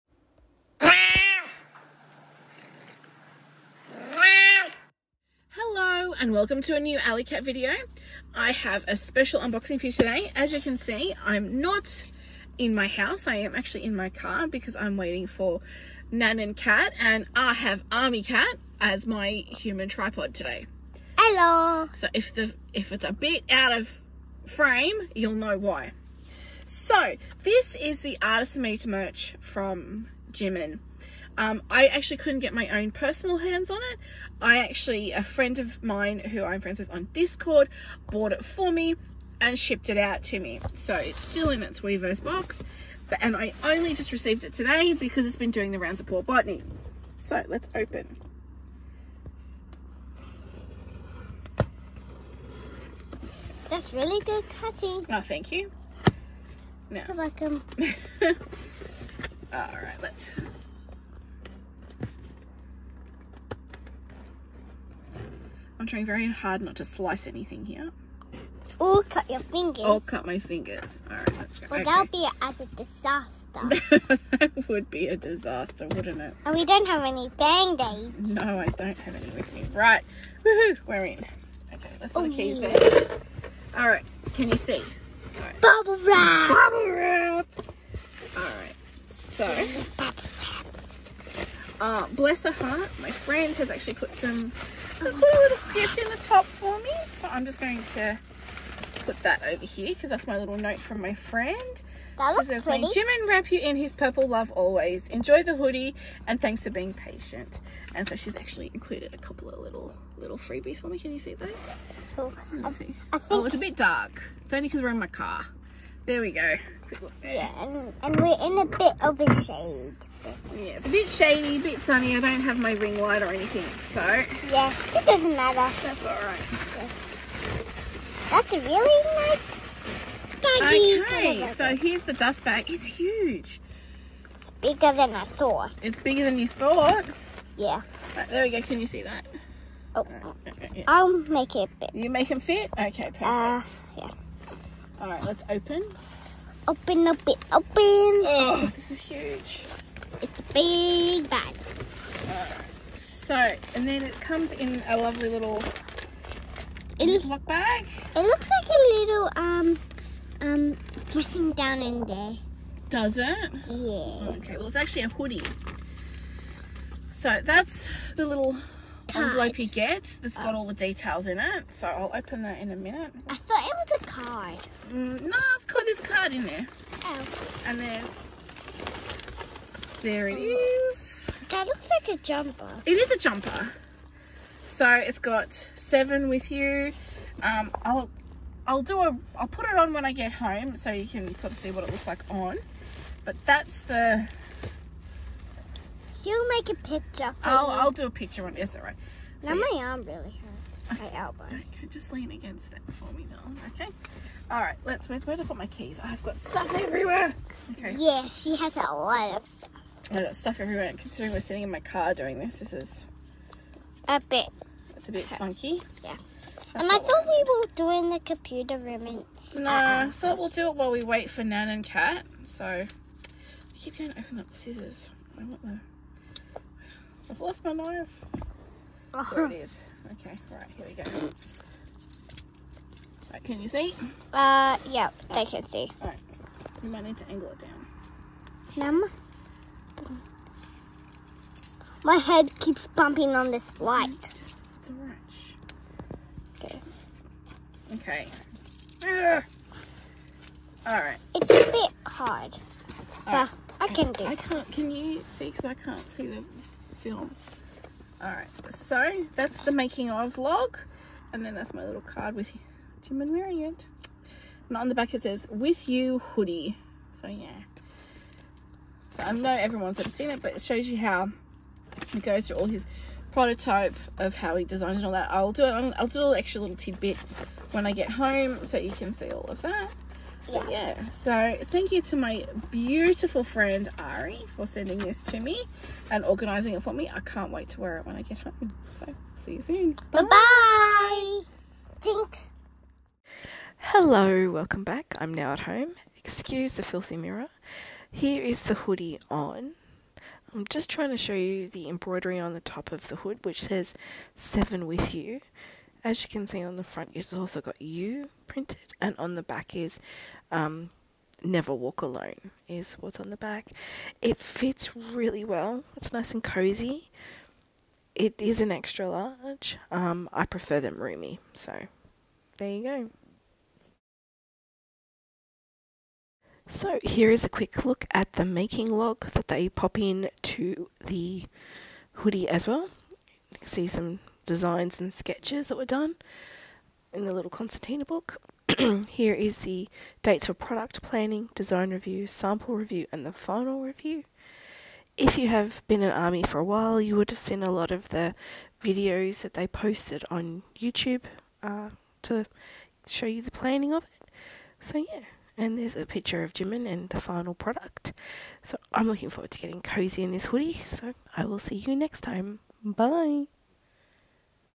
Unboxing Artist Made Merch - Jimin
Come and unbox my Artist Made Merch hoodie by Jimin with me….. it’s so pretty!!